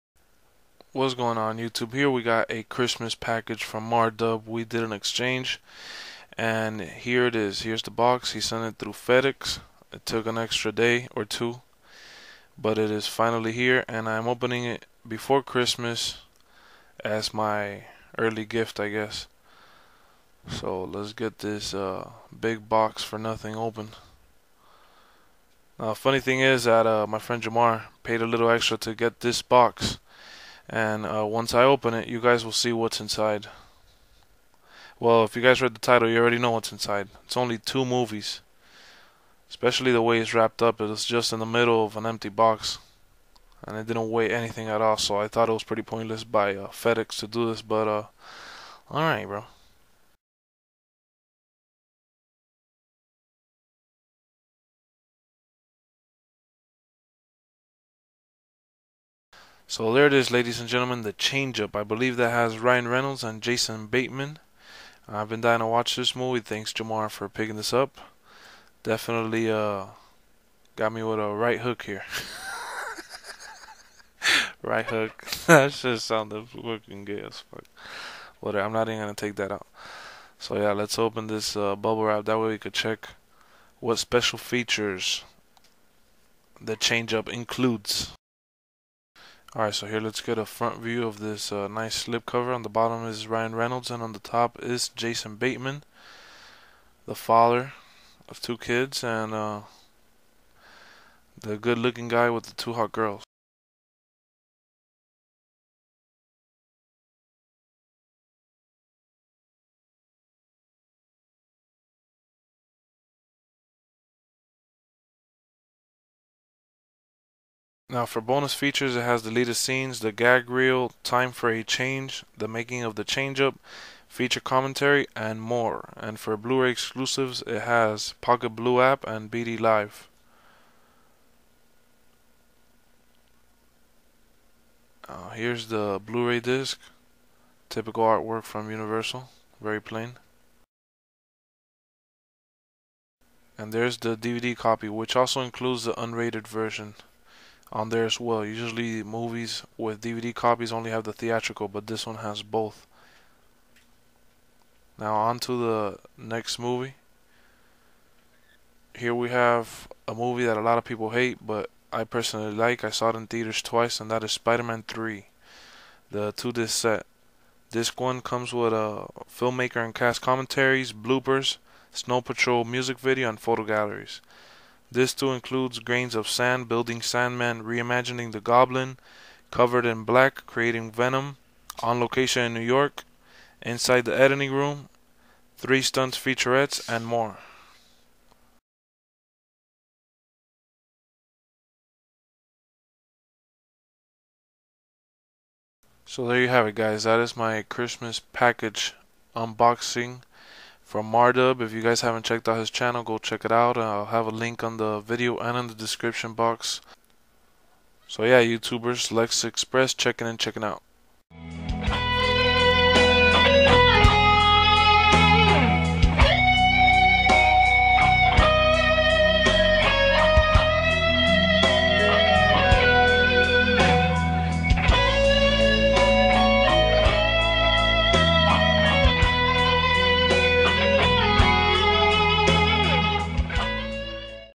Early Christmas Gift: The Change-Up (2011) Unboxing from MarDub (Holiday Gift Exchange)
Jamar aka Mardub and me did a Christmas gift exchange, and here i have the package he sent!

The Change-Up (2011)
Dave is a married man with two kids and a loving wife , and Mitch is a single man who is at the prime of his sexual life. One fateful night while Mitch and Dave are peeing in a fountain when lightning strikes and they switch bodies.

Director: David Dobkin
Writers: Jon Lucas, Scott Moore
Stars: Jason Bateman, Ryan Reynolds and Olivia Wilde

A strange black entity from another world bonds with Peter Parker and causes inner turmoil as he contends with new villains, temptations, and revenge.

Director: Sam Raimi
Writers: Sam Raimi (screenplay), Ivan Raimi (screenplay)
Stars: Tobey Maguire, Kirsten Dunst and Topher Grace